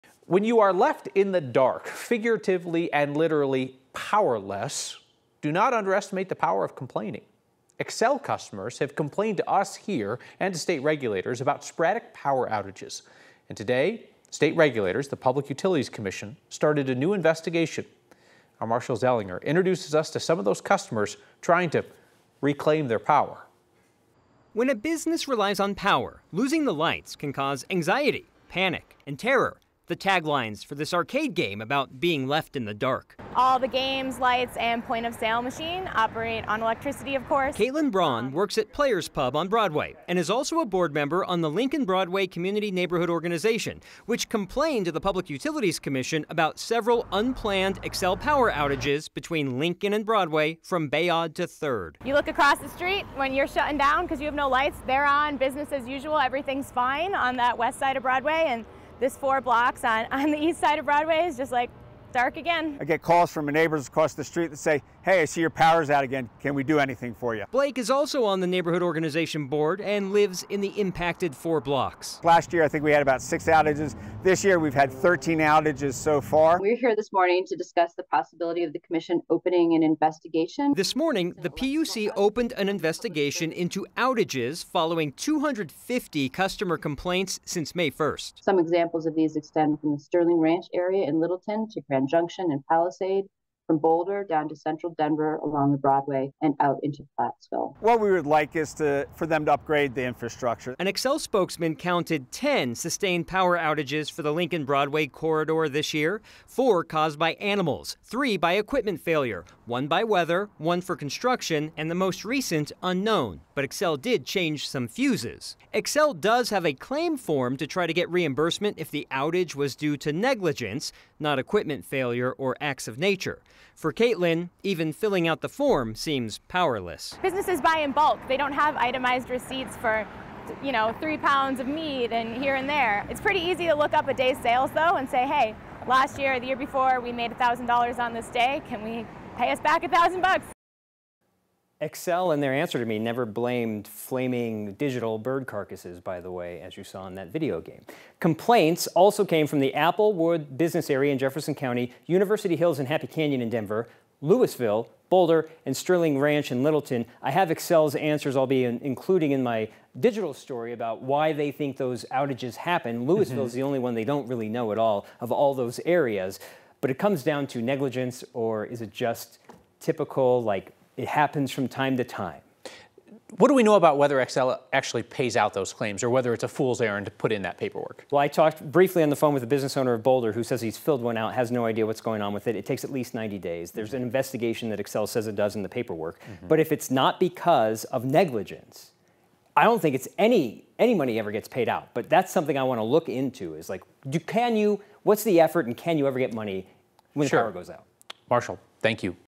PUC will investigate frequent Xcel power outages
The commission agreed to open an investigation following hundreds of complaints of unplanned outages.